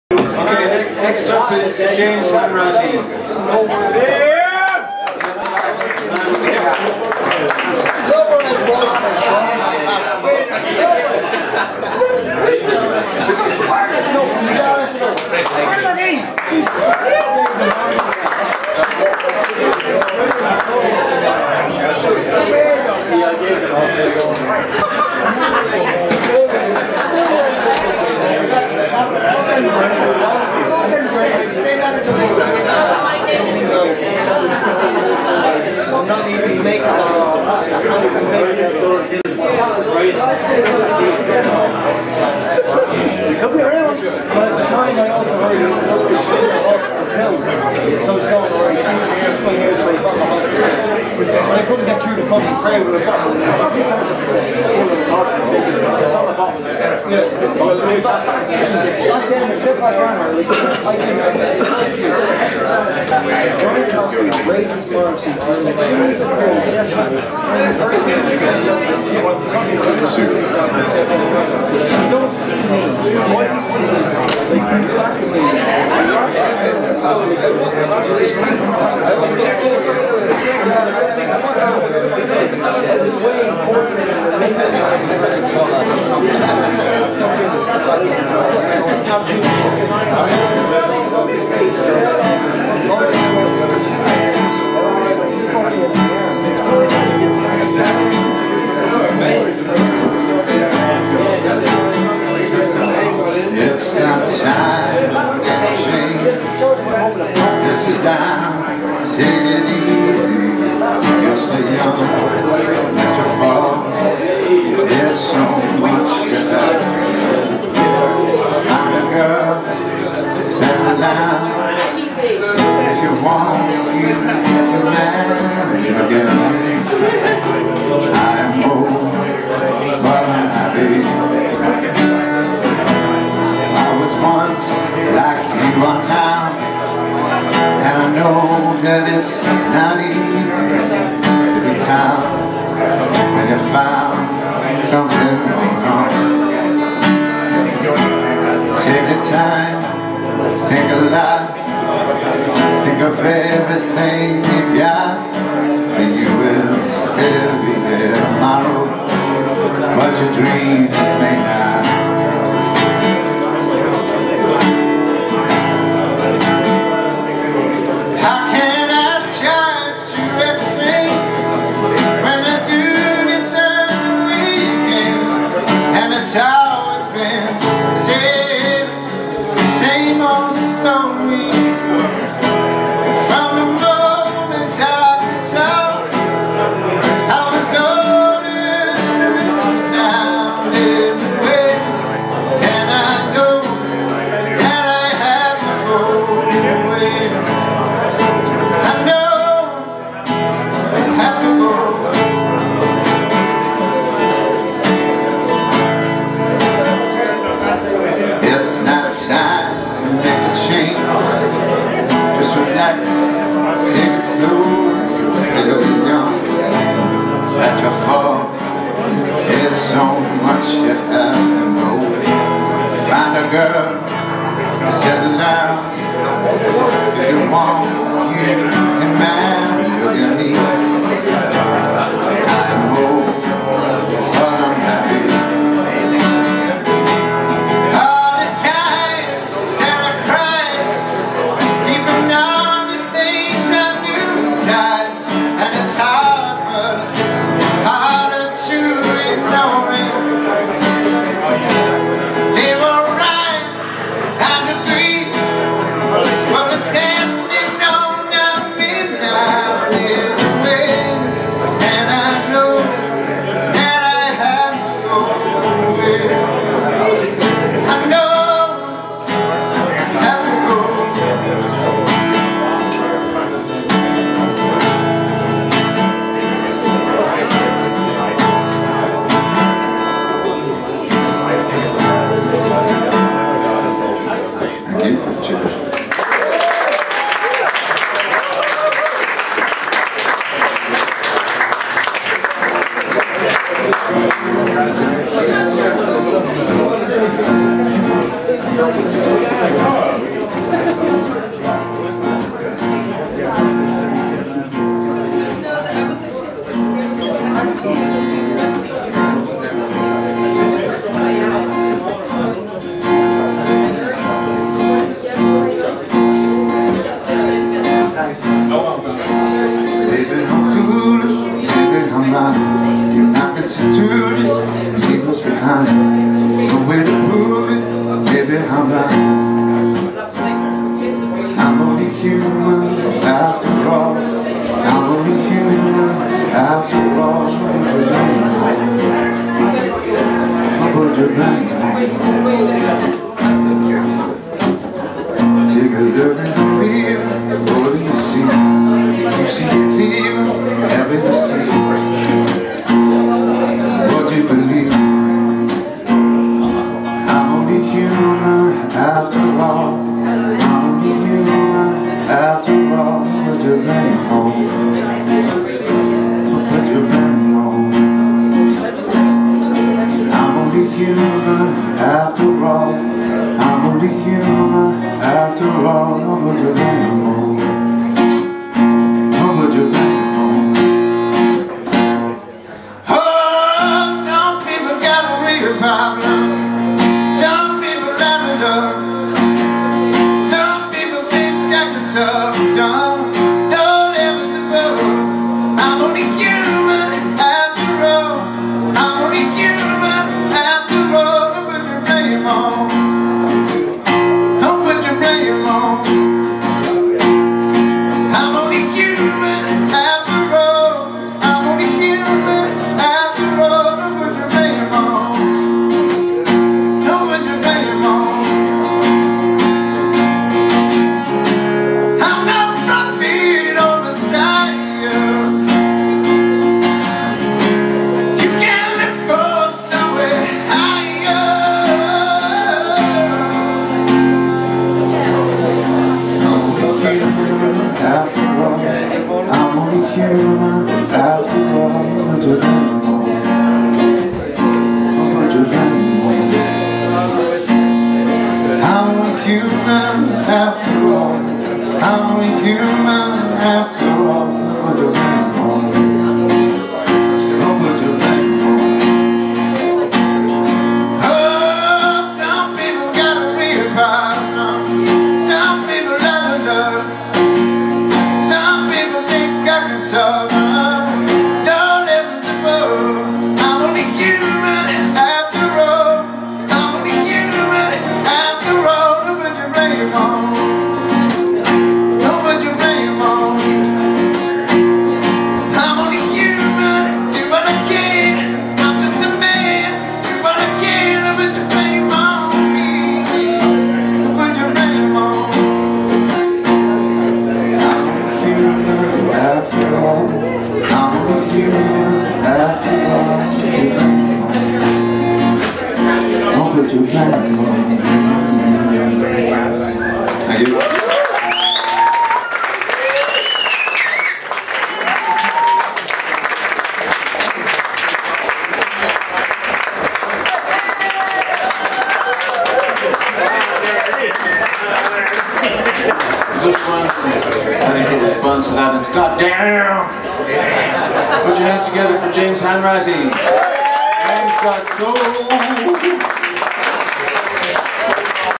James Hanratty 15/02/17
Sea Legs Open Mic Collective, Spirit Store Dundalk, 15/02/2017